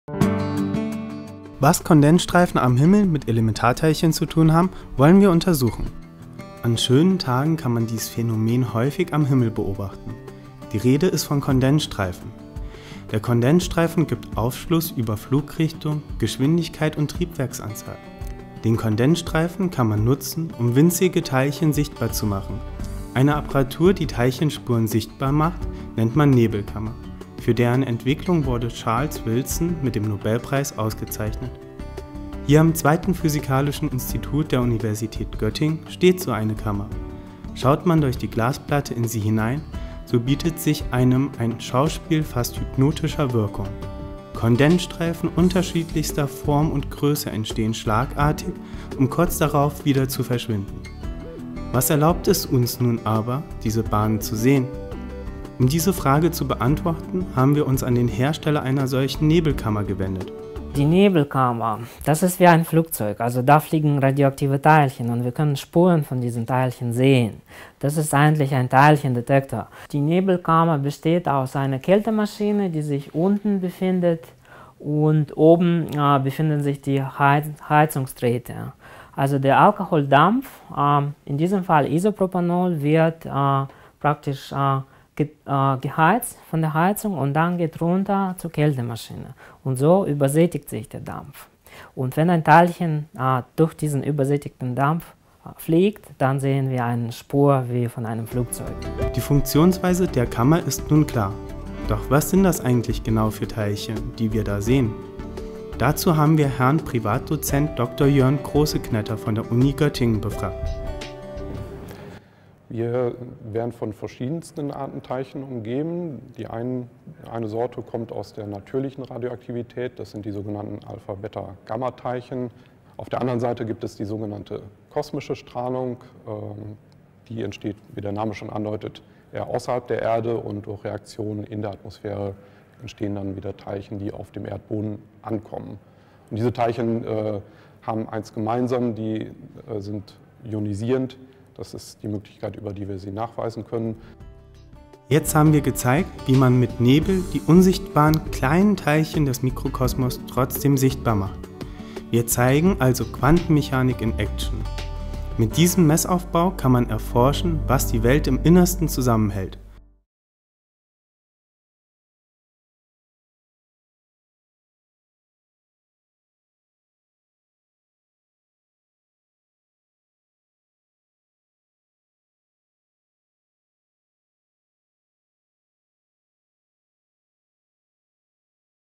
Zauberhafte Physik: Nebelkammer / cloud chamber
Was ist eine Nebelkammer (= cloud chamber) und wie kann man damit Elementarteilchen untersuchen? Die zugrundeliegende Physik ähnelt der Erzeugung von Kondensstreifen durch Flugzeuge in der Atmosphäre. Anhand der Dicke, Richtung und Länge solcher Kondensstreifen kann man Teilcheneigenschaften wie Masse, elektrische Ladung, Flugrichtung und Geschwindigkeit und somit letztlich die Teilchensorte bestimmen. Der Clip zeigt physikalische Phänomen und erläutert die Zusammenhänge.

Ihr interessiert euch für Physik? Hier erfahrt ihr mehr über ein Studium an der Uni Göttingen